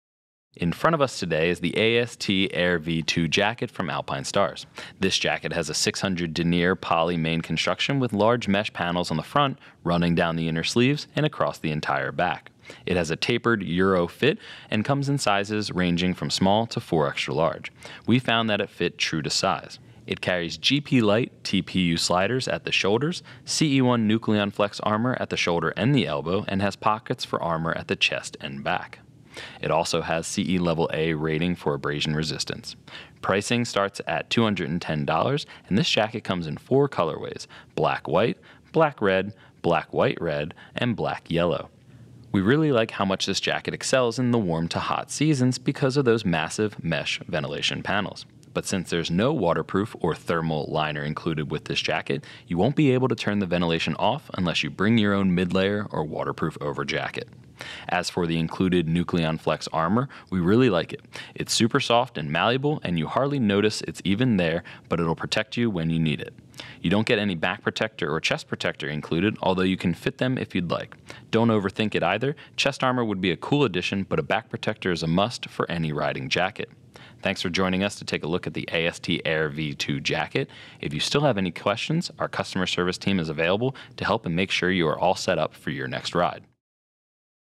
Alpinestars AST Air v2 Jacket Overview
| Alpinestars AST Air v2 Jacket

The Alpinestars AST Air v2 Jacket is an exceptionally light and durable jacket featuring a main mesh shell for superior ventilation. Compatible with the brand's Tech-Air 5 airbag system, the AST Air v2 already comes with 600 Denier poly-fabric and Nucleon Flex protectors in the elbows and shoulders.